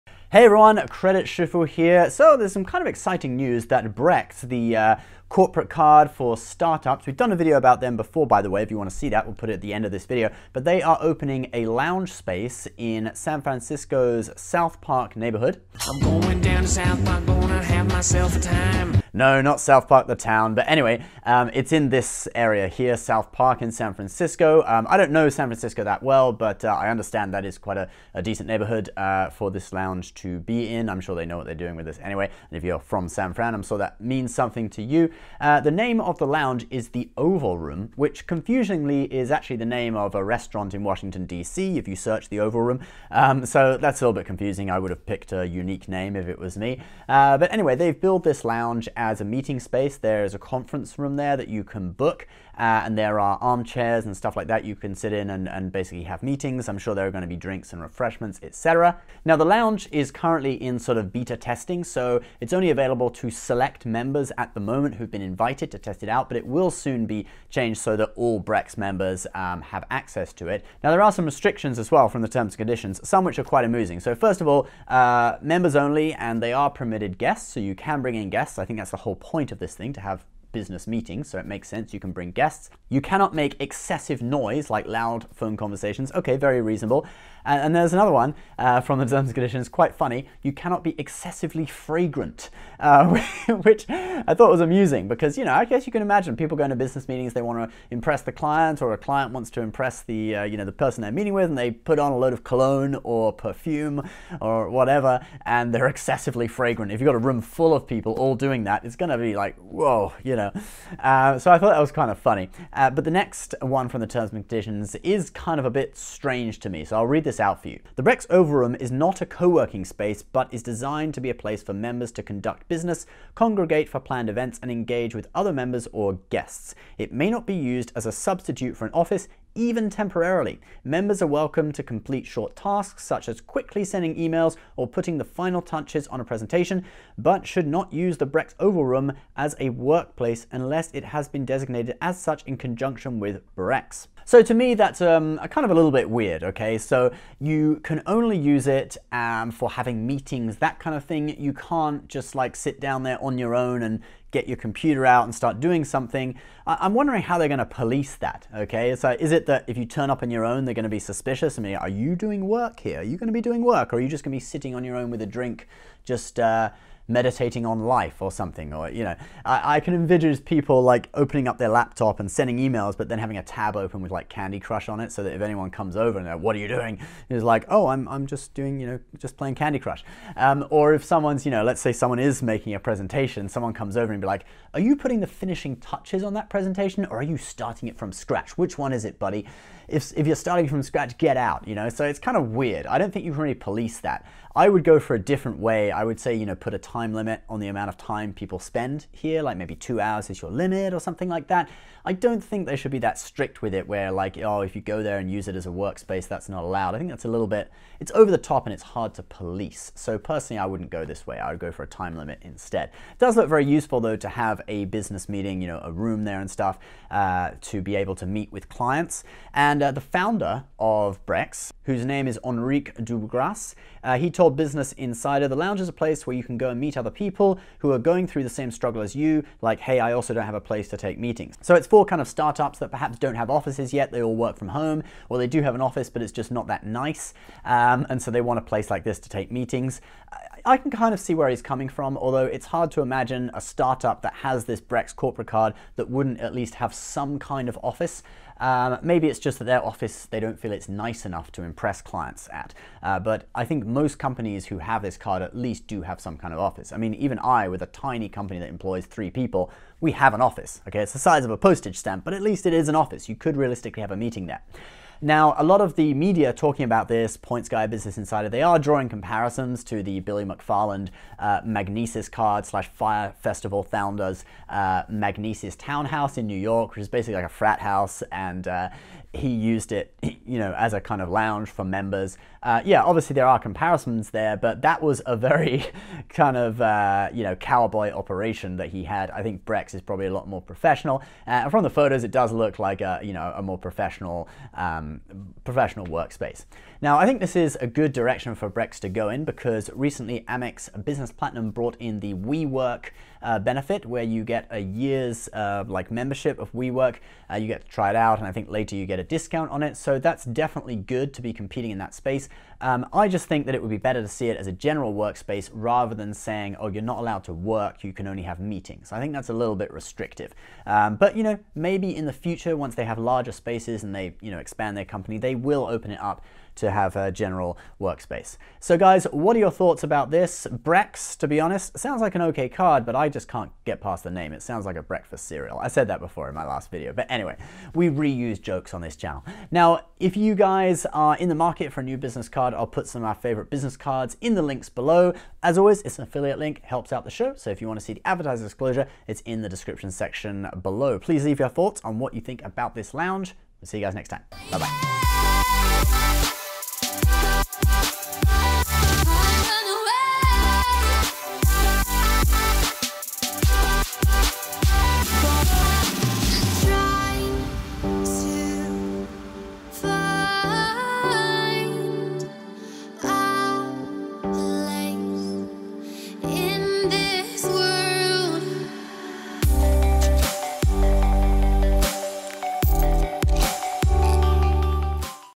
Brex Business Credit Card Opens New Lounge in San Francisco
Click "show more" for ad disclosure

Brex business credit cards have opened a new lounge in San Francisco called the Oval Room.